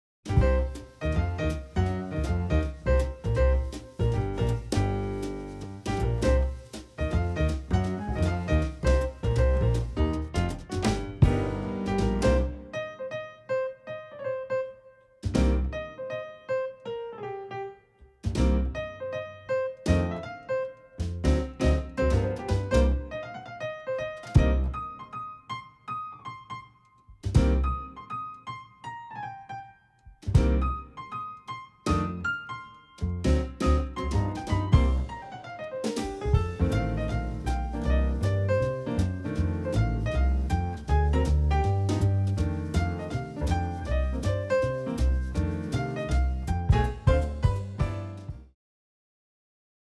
Who needs school supplies when you can have nails like this?
@nails_by_unixo created this stationary-inspired nail art.